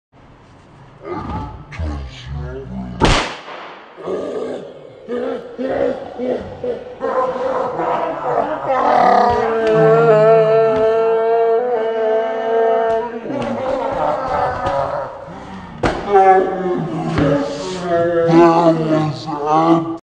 Firecracker in middle of Cigarette
Loaded a cigarette with a blackcat and filled the rest with tobacco for the 4th of july